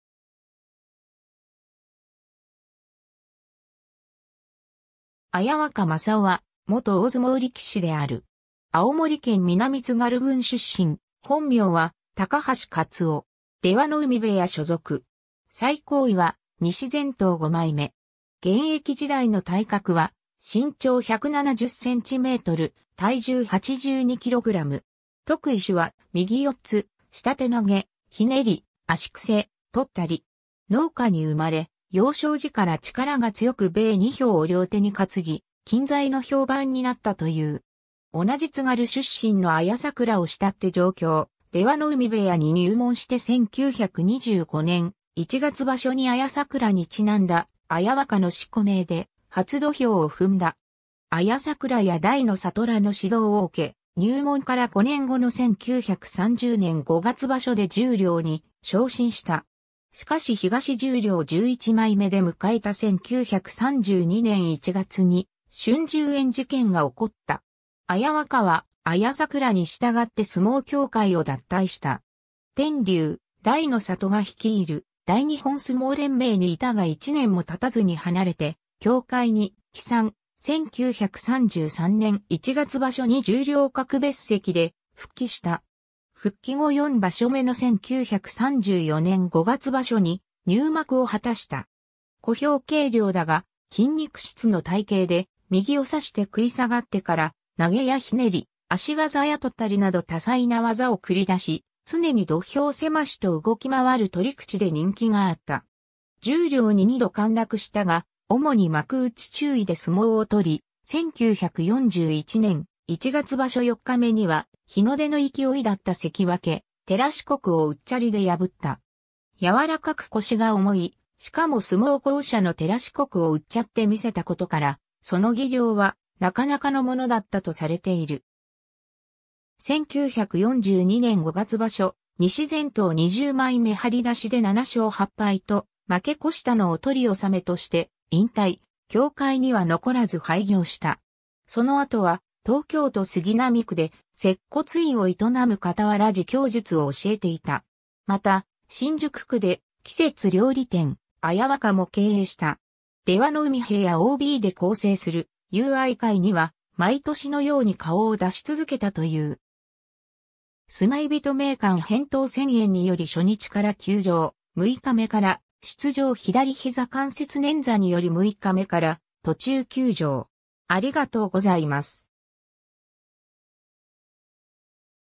綾若真生
綾若 真生（あやわか まさお、1909年11月20日 - 1989年11月10日）は、元大相撲力士である。
青森県南津軽郡（現在の黒石市）出身、本名は 高橋 勝雄（たかはし かつお）。
出羽海部屋所属。
最高位は西前頭5枚目（1940年1月場所）。
現役時代の体格は身長170cm、体重82kg。
得意手は右四つ、下手投げ、捻り、足癖、とったり。
 農家に生まれ、幼少時から力が強く米2俵を両手に担ぎ近在の評判になったという。
同じ津軽出身の綾櫻を慕って上京、出羽海部屋に入門して1925年1月場所に綾櫻に因んだ「綾若」の四股名で初土俵を踏んだ。
綾櫻や大ノ里らの指導を受け、入門から5年後の1930年5月場所で十両に昇進した。
 しかし東十両11枚目で迎えた1932年1月に春秋園事件が起こった。
綾若は綾櫻に従って相撲協会を脱退した。
天竜・大ノ里が率いる大日本相撲連盟（1933年から関西角力協会）にいたが1年もたたずに離れて協会に帰参、1933年1月場所に十両格別席で復帰した（この時の別席番付では「綾若完三郎」の名で最後尾に載っている）。
復帰後4場所目の1934年5月場所に入幕を果たした。
 小兵軽量だが筋肉質の体型で、右を差して食い下がってから投げや捻り、足技やとったりなど多彩な技を繰り出し、常に土俵狭しと動き回る取り口で人気があった。
十両に2度陥落したが主に幕内中位で相撲を取り、1941年1月場所4日目には日の出の勢いだった関脇照國（のち38代横綱）をうっちゃりで破った。
柔らかく腰が重い、しかも相撲巧者の照國をうっちゃって見せたことからその技量はなかなかのものだったとされている。
 1942年5月場所、西前頭20枚目張出で7勝8敗と負け越したのを取り納めとして引退、協会には残らず廃業した。
その後は東京都杉並区で接骨院を営むかたわら自彊術を教えていた。
また、新宿区で季節料理店「綾若」も経営した。
出羽海部屋OBで構成する「友愛会」には毎年のように顔を出し続けたという。
 ^ 相撲人名鑑（綾若 真生） ^ 扁桃腺炎により初日から休場、6日目から出場 ^ 左膝関節捻挫により6日目から途中休場